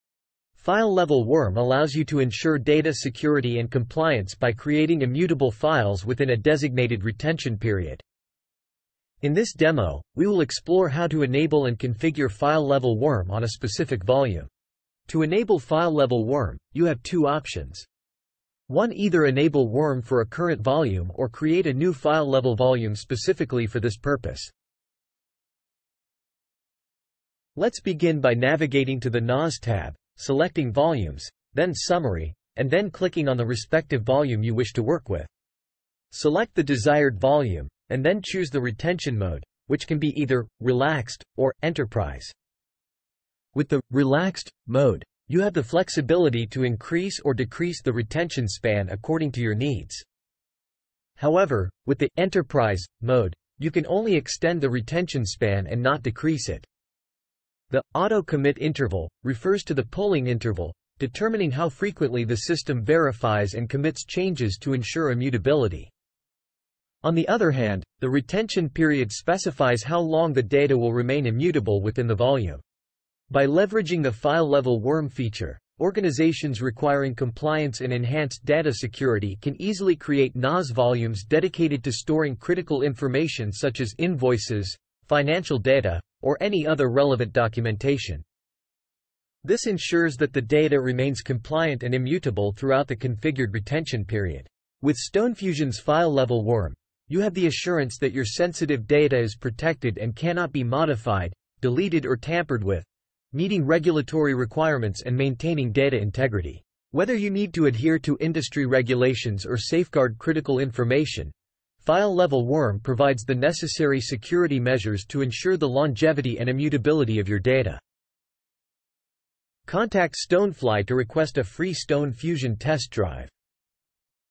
How to Enable File-Level WORM (Immutable Storage) | StoneFusion Demo
In this instructional video, we provide a step-by-step guide on how to set up file-level Write-Once-Read-Many (WORM) functionality using our advanced storage operating system, StoneFusion. Learn how to implement a secure and immutable storage solution that prevents unauthorized modification or deletion of critical files. Follow along as we demonstrate the configuration process, highlighting the key features and benefits of file-level WORM for enhanced data protection and compliance.